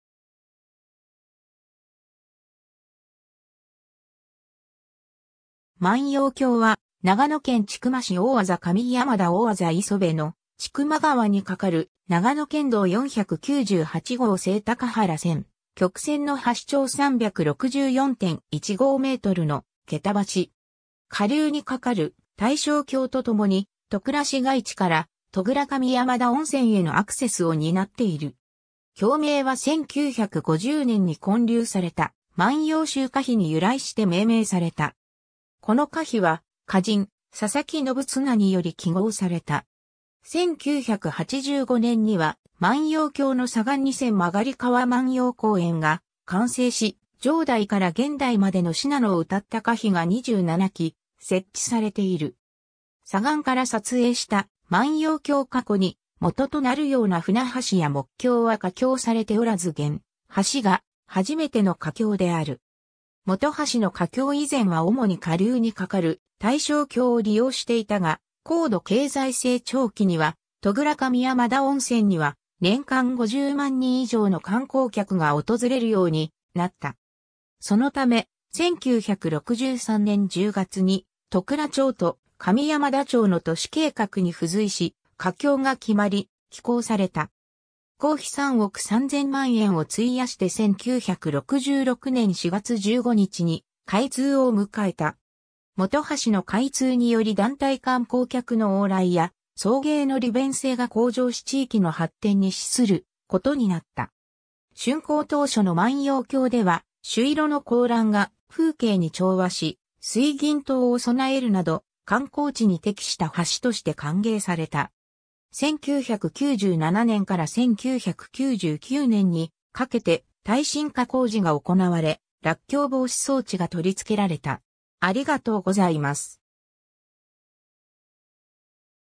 万葉橋
万葉橋（まんようばし）は、長野県千曲市大字上山田 - 大字磯部の千曲川に架かる長野県道498号聖高原千曲線の橋長364.15メートル (m) の桁橋。
 下流に架かる大正橋と共に戸倉市街地から戸倉上山田温泉へのアクセスを担っている。
橋名は1950年（昭和25年）に建立された万葉集歌碑に由来して命名された。
この歌碑は歌人佐佐木信綱により揮毫された。
1985年（昭和60年）には万葉橋の左岸に千曲川万葉公園が完成し、上代から現代までの信濃を歌った歌碑が27基設置されている。
 左岸から撮影した万葉橋 過去に元となるような舟橋や木橋は架橋されておらず現橋が初めての架橋である。
本橋の架橋以前は主に下流に架かる大正橋を利用していたが、高度経済成長期には戸倉上山田温泉には年間50万人以上の観光客が訪れるようになった。
そのため、1963年（昭和39年）10月に戸倉町と上山田町の都市計画に付随し架橋が決まり、起工された。
工費3億3000万円を費やして1966年（昭和41年）4月15日に開通を迎えた。
本橋の開通により団体観光客の往来や送迎の利便性が向上し地域の発展に資することになった。
竣工当初の万葉橋では朱色の高欄が風景に調和し、水銀灯を備えるなど観光地に適した橋として歓迎された。
 1997年（平成9年）から1999年（平成11年）にかけて、耐震化工事が行われ、落橋防止装置が取り付けられた。